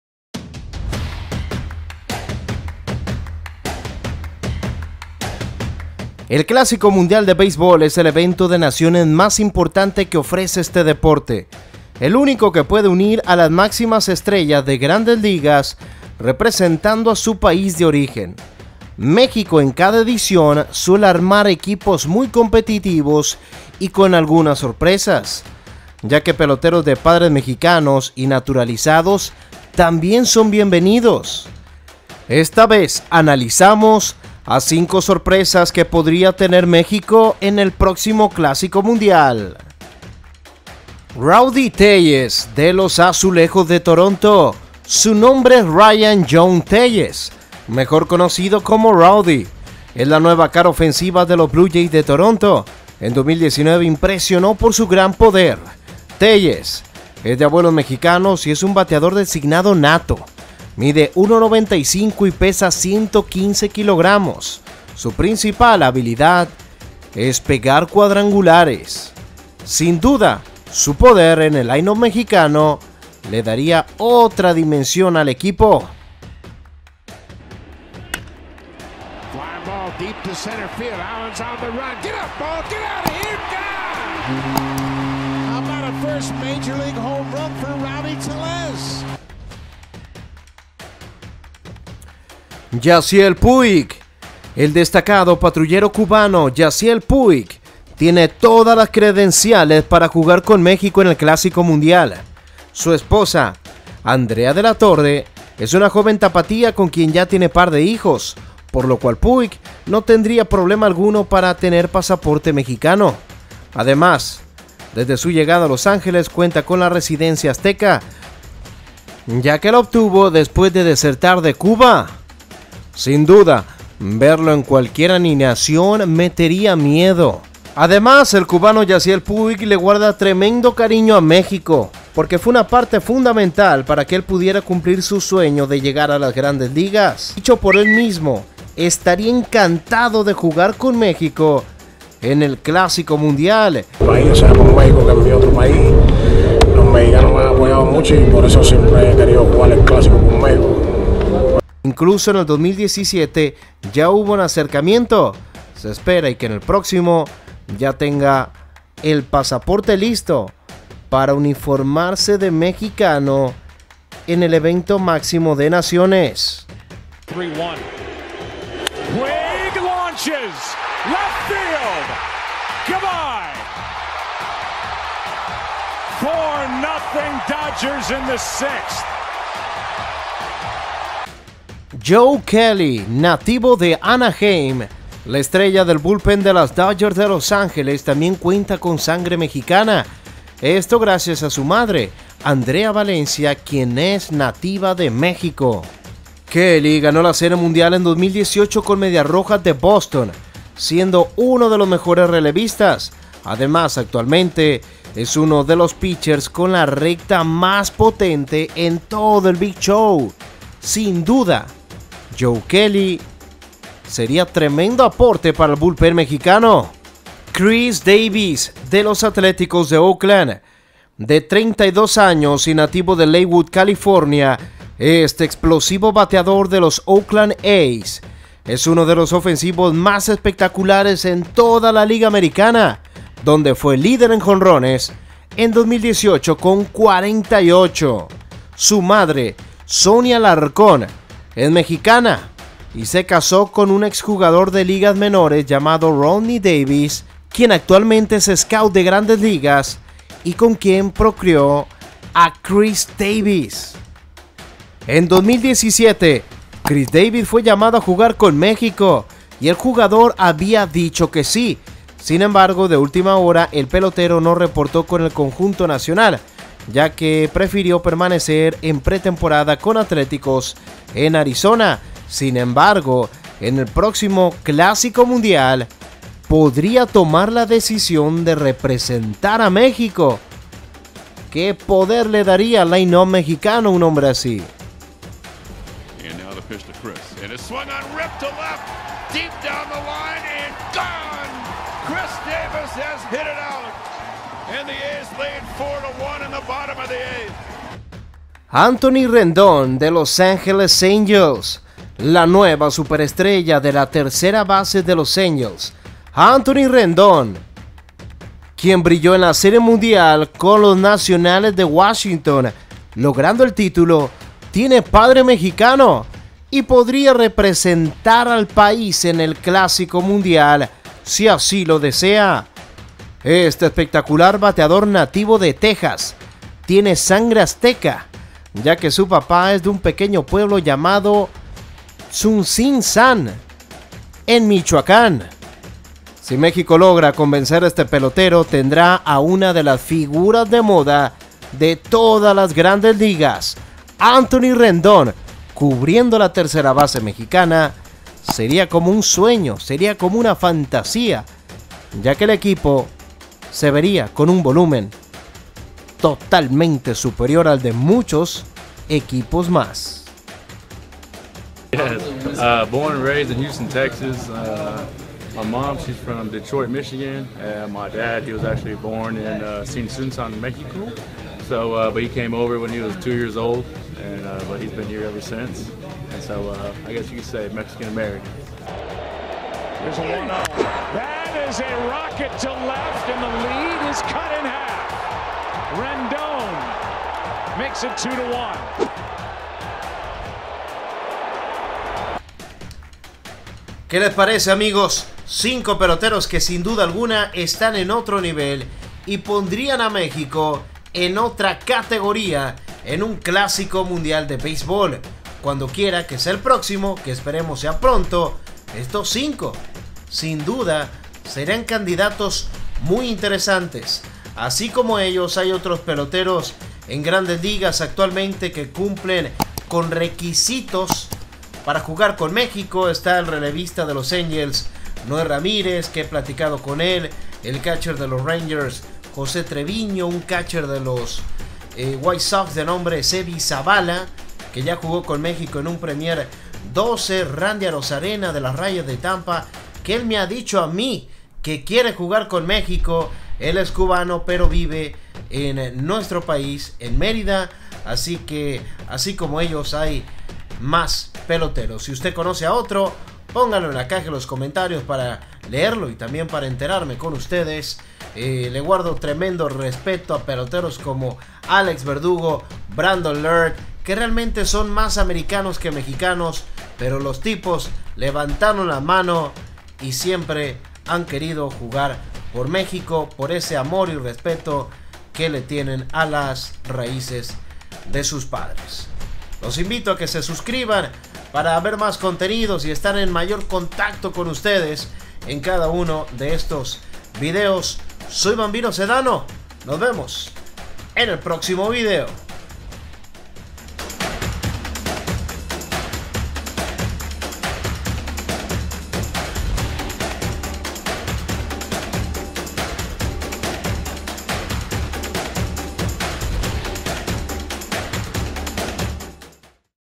TOP 5 🔥 SORPRESAS DE MÉXICO PARA EL PRÓXIMO CLÁSICO MUNDIAL
¿EQUIPO SOÑADO? 🌝🇲🇽

México podría tener sorpresas espectaculares para el próximo Clásico Mundial de Beisbol, elementos que actualmente brillan en las Grandes Ligas.